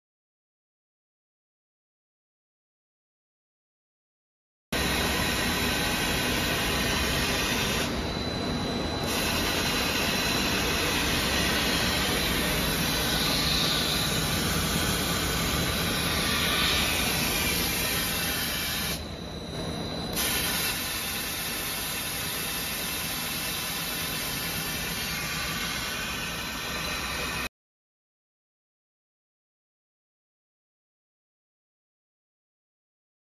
Автоматический одноосный станок для резки рулонов серии LGQT
Предлагаем вашему вниманию видео работы автоматического одноосного станка. С полным описанием и характеристиками вы можете ознакомится, посетив наш сайт